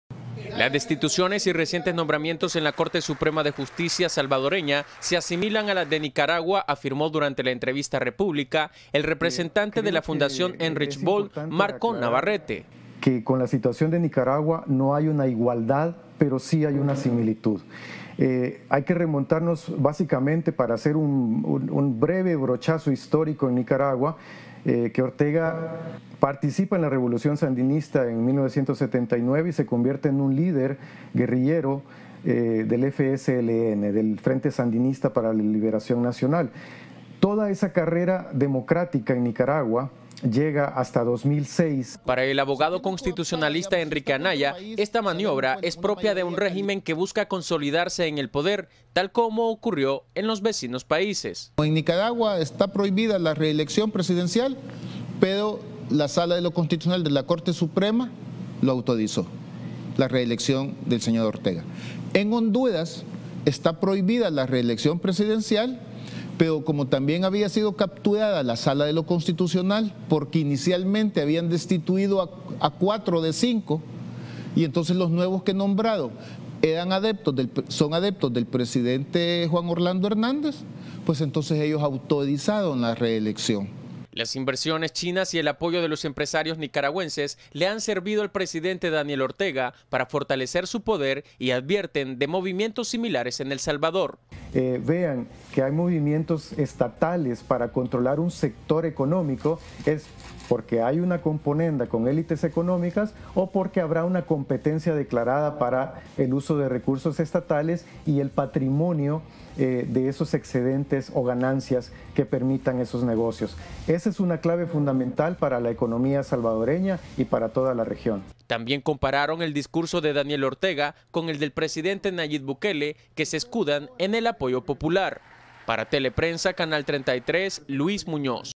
Teleprensa33 | Comparan sistema judicial de El Salvador con el de Nicaragua
El representante de la Fundación Heinrich Böll, Marco Navarrete explicó algunas similitudes que ha alcanzado el sistema judicial de El Salvador con el de Nicaragua.

Jueves, 8 de julio de 2021